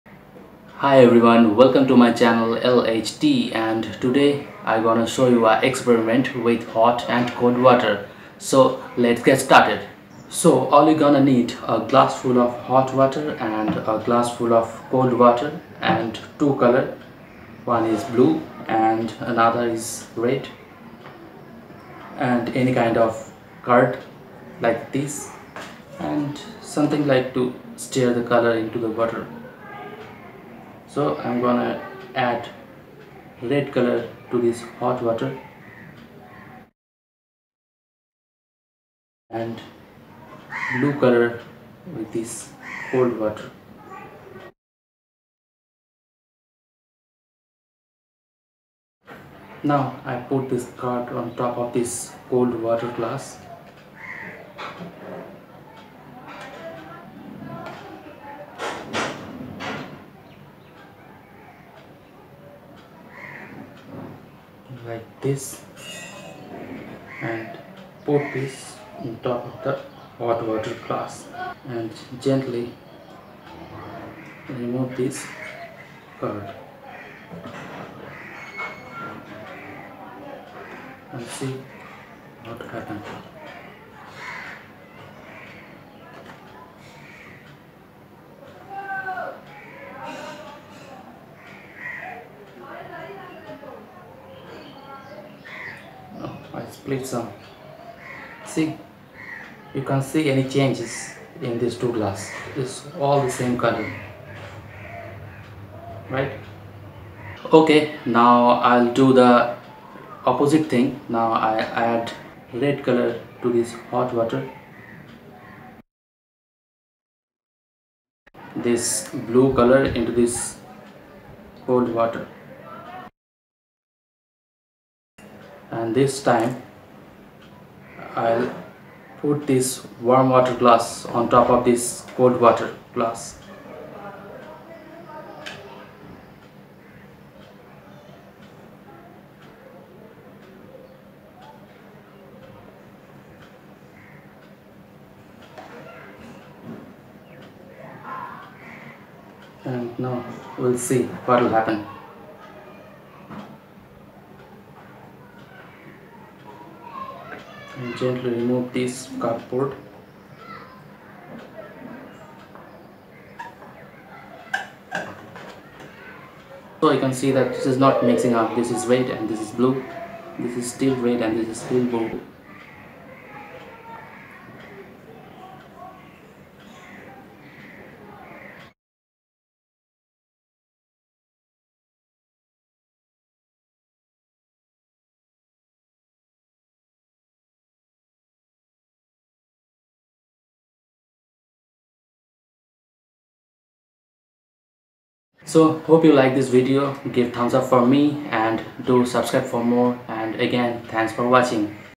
Hot and Cold Water Experiment | Experiment Project With Cold And Hot Water
This experiment takes advantage of the effect of temperature on density. A colder liquid is more dense than a warmer liquid. The molecules in cold water move around less than the molecules in hot water. They have less energy to use for motion because the temperature is lower. Because the cold water molecules move less, more molecules can be packed into the same total volume than in the case of hot water. This make cold water more dense than hot water. Because of the difference in density, the hot water tends to float on top of the cold water rather than mix with it. This is why the colors seems to stay separate. The longer the hot and cold water are left in contact, the closer in temperature they become. Then the red blue water begin to mix.